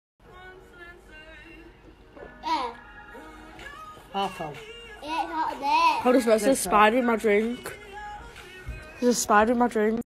Woman notices giant spider inside cup
Paige from the United Kingdom is drinking her beer when she notices a giant spider floating around inside her cup. Unfortunately, Paige drinks from the cup with the spider in it before realizing.